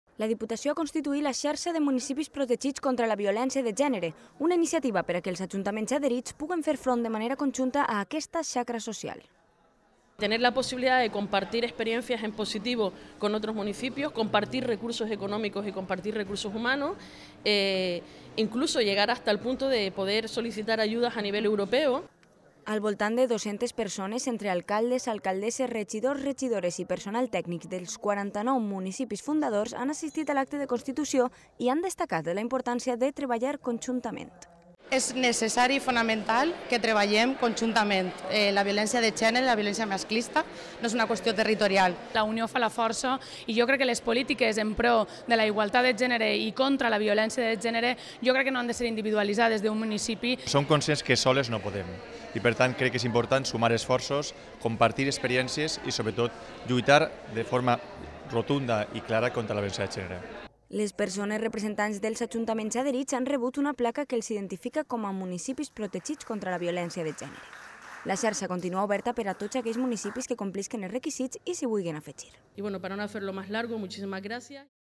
La Diputació constitueix una Xarxa de municipis per a lluitar contra la violència de gènere
Donar la millor resposta possible contra la violència de gènere. Aquest és l'objectiu de la Diputació de València, que al llarg de la present legislatura treballa intensament des de l'àrea d'Igualtat en programes i iniciatives amb les quals ajudar els ajuntaments a combatre la xacra social de la violència masclista. L'última d'aquestes és la constitució de la Xarxa de Municipis Protegits contra la Violència de Gènere, que aquest dimecres veu la llum després de dos anys d'intens treball institucional per a posar-la en marxa